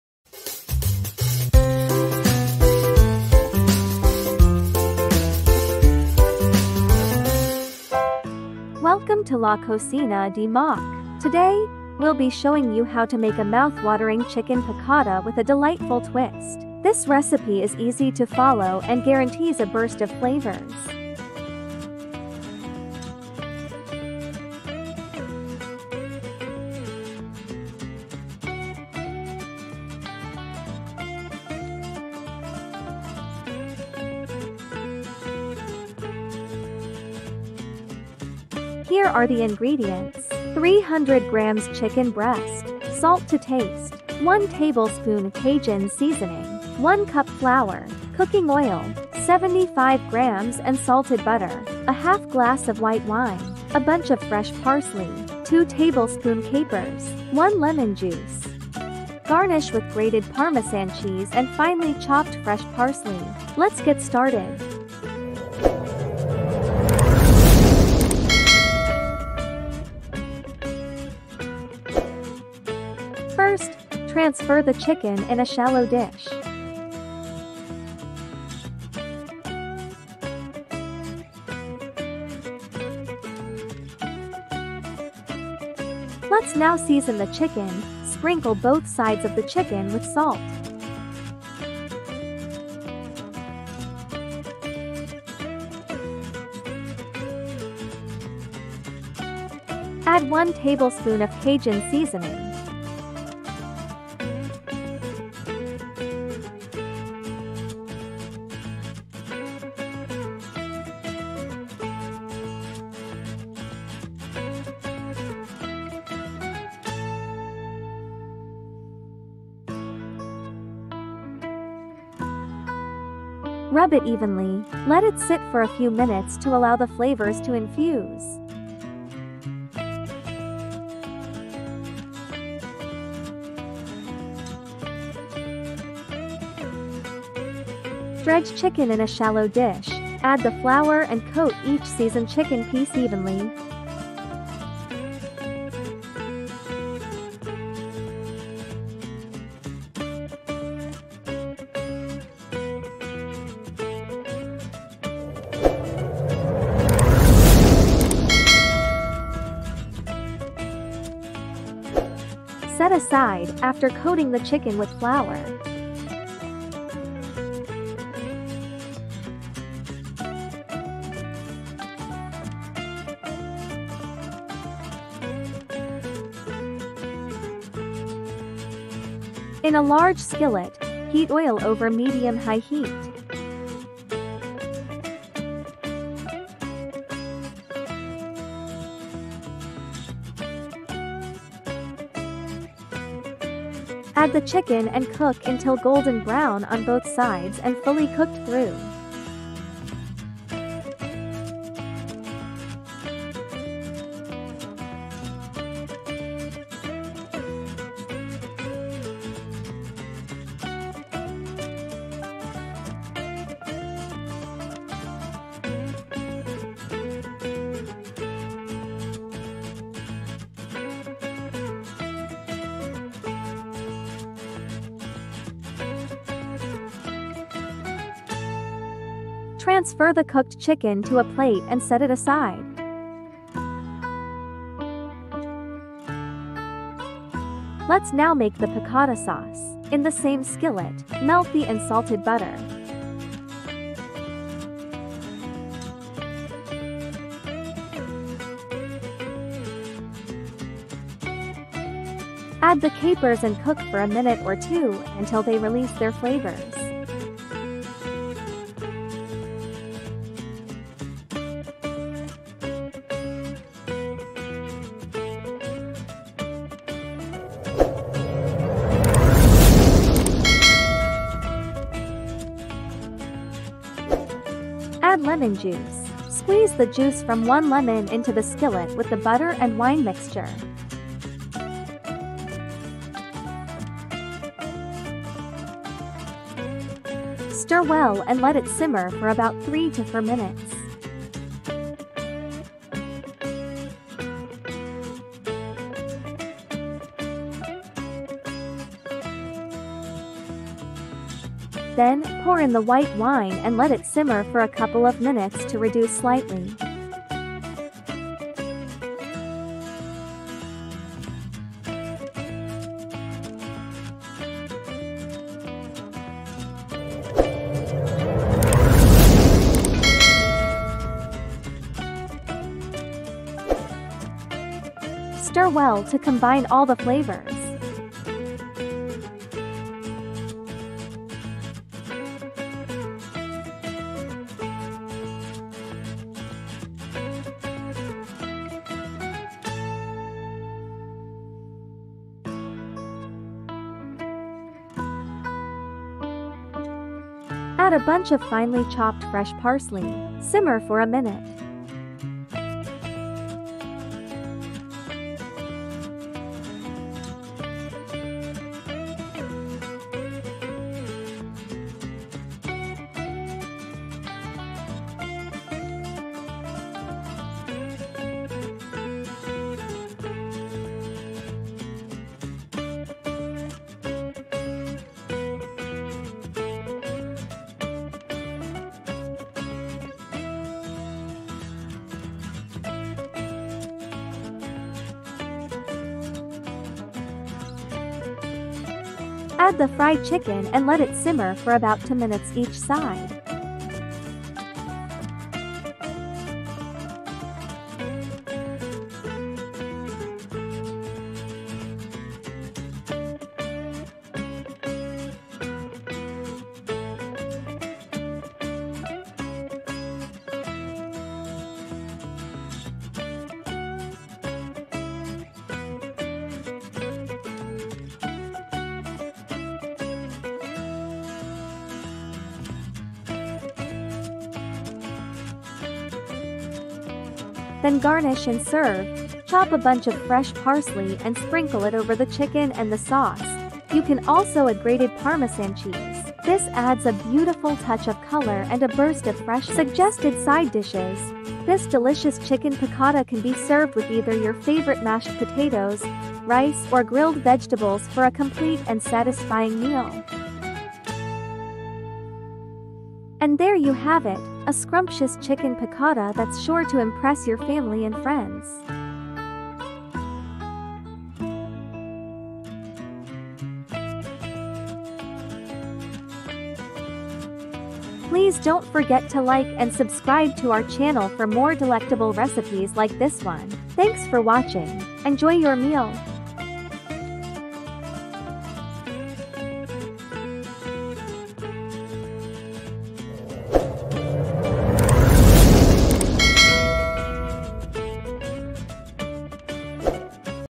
Delicious Chicken Piccata Recipe
👋Welcome to La Cocina de MOK! Today, we'll be showing you how to make a mouthwatering Chicken Piccata with a delightful twist. This recipe is easy to follow and guarantees a burst of flavors.

👋 ¡Bienvenidos a La Cocina de MOK! Hoy, les mostraremos cómo hacer un apetitoso Pollo Piccata con un toque encantador. Esta receta es fácil de seguir y garantiza una explosión de sabores.

✍️Here are the Ingredients:

- 300 grams chicken breast
- Salt to taste
- 1 tablespoon Cajun seasoning
- 1 cup flour
- Cooking oil
- 75 grams unsalted butter
- 1/2 glass of white wine
- A bunch of fresh parsley
- 2 tablespoon capers
- 1 lemon juice

Garnish with grated Parmessan cheese (optional) and finely chopped fresh parsely.

Intro Music from youtube library